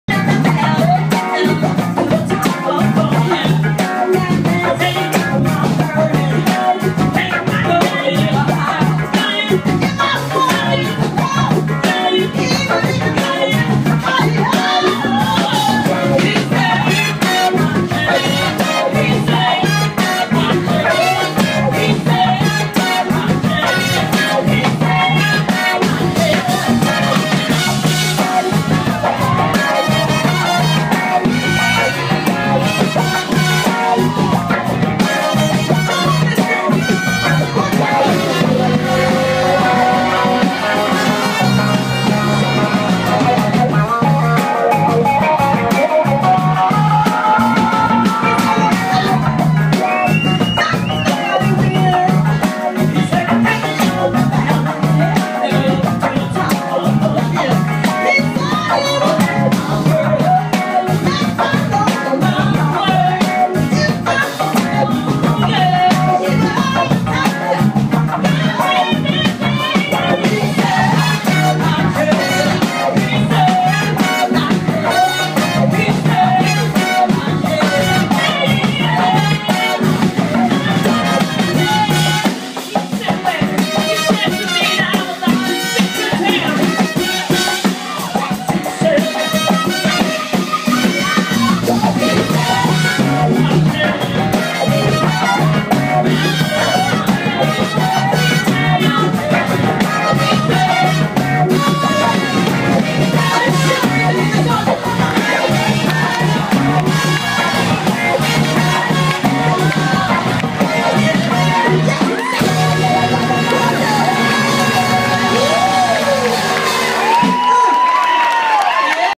Sharon jones pulls me up to sing with her on her new song a
The corner hotel with Sharon jones 2013!